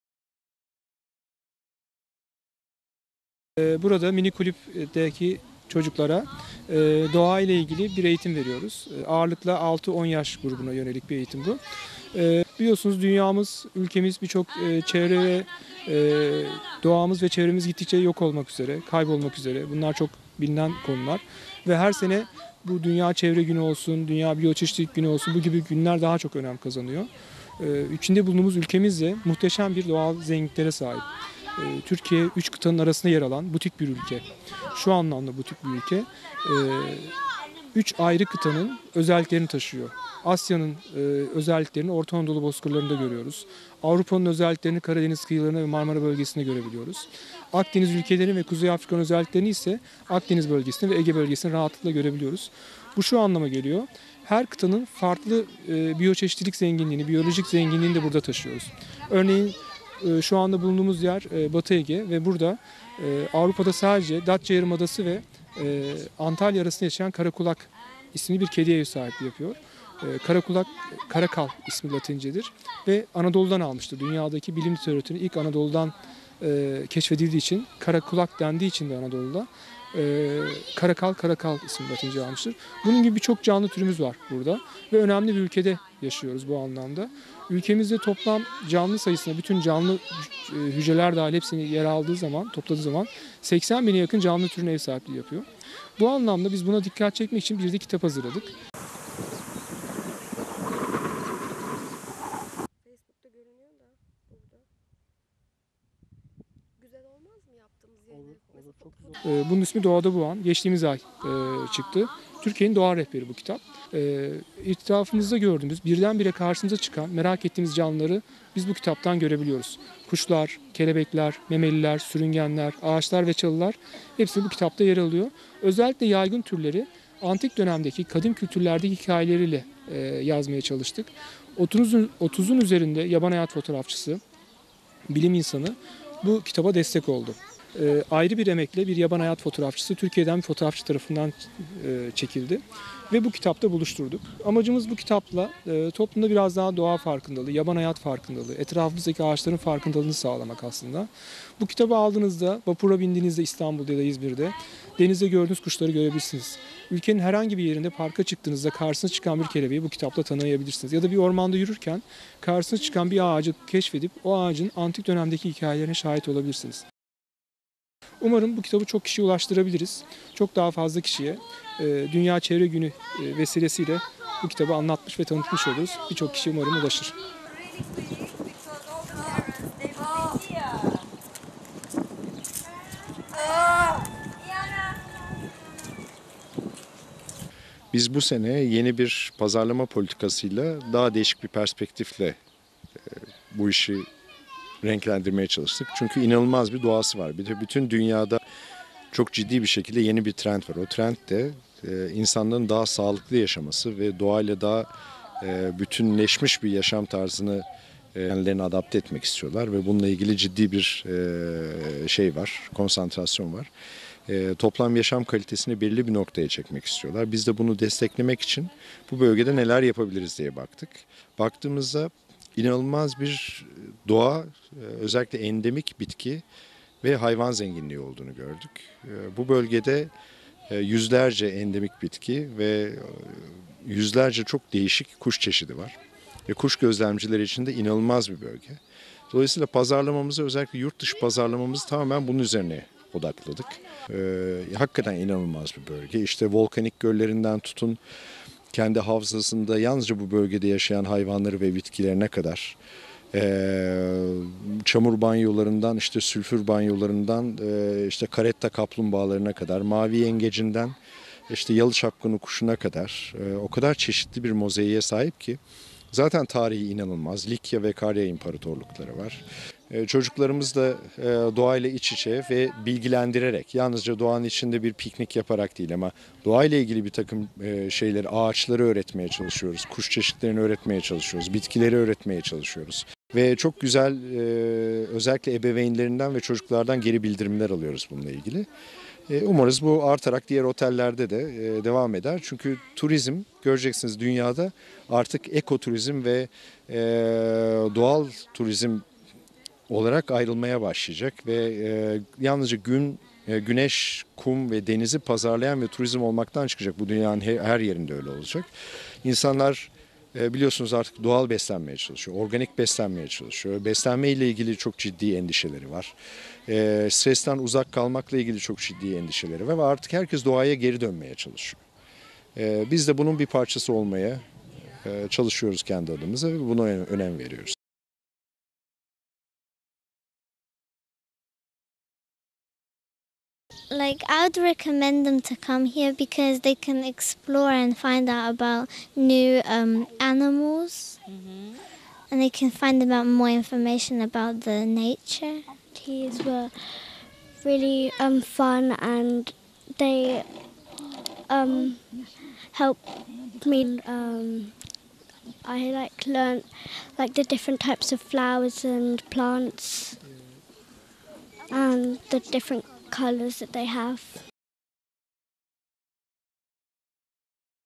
HİLTON'DAN ÖNEMLİ ÇALIŞMA
Ortaca Sarıgerme’deki Hilton Dalaman Sarıgerme Resort & Spa’da tatillerini geçiren ailelerin çocukları Biyomühendis Hüseyin Çağlar İnce ile doğa gezisine çıktı.
5 Haziran Dünya Çevre Günü’nde çocuklar doğayla baş başa bir gün geçiren minik misafirler Hüseyin Çağlar İnce’nin geçtiğimiz günlerde çıkan ve raflarda yerini “Doğadabuan” adlı kitabında topladığı yörede yetişen endemik bitki türlerini ve canlıları incelediler.
Batı Toroslar'ın Göktepe ve Yaylacık dağlarından inen kolların birleşmesiyle büyüyen, Ege ile Akdeniz’i birbirinden ayıran, toplam uzunluğu 229 kilometre Antik ismi İndos olan Dalaman çayının kıyılarında kuş gözlemciliği yapan çocuklar unutulmaz anlar yaşadılar.

Biyomühendis Hüseyin Çağlar İnce,  Hilton Dalaman Sarıgerme Resort & Spa bünyesinde kurulan mini kulüpte 6-10 yaş grubuna yönelik doğa ile ilgili eğitim verdiklerini söyledi.

-Her Kıta’nın biyolojik ve biyoçeşitlilik zenginliği Türkiye’de
Türkiye’nin üç kıtanın arasında yer aldığına işaret eden Biyomühendis İnce, “Doğamız ve çevremiz gittikçe yok olmak üzere, bunlar çok bilinen konular. Her sene dünya çevre günü olsun, dünya biyo çeşitlilik günü olsun bu gibi günler daha çok önem kazanıyor. İçinde bulunduğumuz ülkemizde muhteşem bir doğal güzelliklere sahip. Türkiye üç kıtanın arasında yer alan butik bir ülke, üç ayrı kıtanın özelliklerini taşıyor. Asya’nın özelliklerini Orta Anadolu’da görüyoruz. Avrupa’nın özelliklerini Karadeniz kıyılarında ve Marmara bölgesinde görebiliyoruz. Akdeniz ülkelerinin ve Kuzey Afrika’nın özelliklerini ise Akdeniz bölgesinde ve Ege bölgesinde rahatlıkla görebiliyoruz. Bu şu anlama geliyor. Her Kıta’nın farklı biyoçeşitlilik zenginliğini, biyolojik zenginliğini de burada taşıyoruz. Ülkemiz 80 bine yakın canlı türüne ev sahipliği yapıyor” dedi.

-“Doğadabuan” kitapçılarda…
Biyoçeşitlilik ve biyolojik zenginliğe dikkat çekmek için bir kitap hazırladığını ve geçtiğimiz ay çıktığını anlatan İnce, “Bu kitap Türkiye’nin doğa rehberi. Etrafımızda gördüğümüz, birden bire karşımıza çıkan, merak ettiğimiz canlıları biz bu kitapta görebiliyoruz. Kuşlar, kelebekler, memeliler, ağaçlar ve çalılar. Hepsi bu kitapta yer alıyor. Özellikle yaygın türleri antik dönemdeki kadın kültürlerdeki hikayeleriyle yazmaya çalıştık. 30’un üzerinde yaban hayatı fotoğrafçısı, bilim insanı bu kitaba destek oldu. Amacımız bu kitapla toplumda biraz daha doğa farkındalığı, yaban hayat farkındalığı, etrafımızdaki ağaçların farkındalığını sağlamak. Bu kitabı aldığınızda, denizde gördüğünüz kuşları görebilirsiniz. Ülkenin herhangi bir yerinde parka çıktığınızda karşınıza çıkan bir kelebeği bu kitapta tanıyabilirsiniz. Ya da bir ormanda yürürken karşınıza çıkan bir ağacı keşfedip o ağacın antik dönemlerdeki hikayelerine şahit olabilirsiniz. Umarım bu kitabı çok kişiye ulaştırabiliriz” diye konuştu.

-Turizmde yeni trend, eko turizm ve doğal turizm
Hilton Dalaman Sarıgerme Resort & Spa Genel Müdürü Tunç Batum ise, bölgede yüzlerce endemik bitki, yüzlerce değişik kuş çeşidi olduğunu ifade etti.

Yeni bir pazarlama politikasıyla, daha değişik bir perspektifle bu işi renklendirmeye çalıştıklarını kaydeden Tunç Batum, “Bütün dünyada çok ciddi bir şekilde yeni bir trend var. Bu trend de insanların sağlıklı yaşaması, doğayla daha bütünleşmiş bir yaşam tarzını kendilerine adapte etmek istiyorlar. Bizler de bunu araştırdık, bölgemizin inanılmaz bir doğası var. Bununla ilgili ciddi bir konsantrasyon var. Biz de bu konuda bunu desteklemek için bu bölgede neler yapabiliriz diye baktığımızda inanılmaz bir doğa, özellikle endemik bitki ve hayvan zenginliği olduğunu gördük. Kuş gözlemcileri için de inanılmaz bir bölge. Özellikle yurt dışı pazarlamamızı bunun üzerine odakladık. Çünkü turizm dünyada eko turizm ve doğal turizm olarak ayrılmaya başlayacak ve yalnızca güneş, kum ve denizi pazarlayan turizm olmaktan çıkacak. Bu dünyanın her yerinde öyle olacak. Herkes doğaya geri dönmeye çalışıyor. Biz de bunun bir parçası olmaya çalışıyoruz kendi adımıza önem veriyoruz” dedi.

-Misafir çocuklara doğa bilinci
Otel bünyesinde oluşturulan ekip ile otelde kalan misafirlerin çocuklarına doğa bilincini aşılamak adına çeşitli aktiviteler yaptıklarını da belirten Batum, “Sadece doğanın içinde piknik yaparak değil, doğa ile bir takım şeyleri, ağaçları öğretmeye çalışıyoruz, kuş çeşitlerini öğretmeye çalışıyoruz, bitkileri öğretmeye çalışıyoruz. Çocuklarımızla doğa ile iç içe yaşayarak bilgilendiriyoruz. Ebeveynlerinden ve çocuklardan çok güzel geri bildirimler. Umarız bu artarak diğer otellerde de devam eder”

-Etkinliğe katılan çocuklar ise düşüncelerini, doğa içerisinde animasyonlarla dolu dolu bir gün geçirdiklerini ve çok mutlu olduklarını dile getirdiler.
ÖMER KUNDAKÇI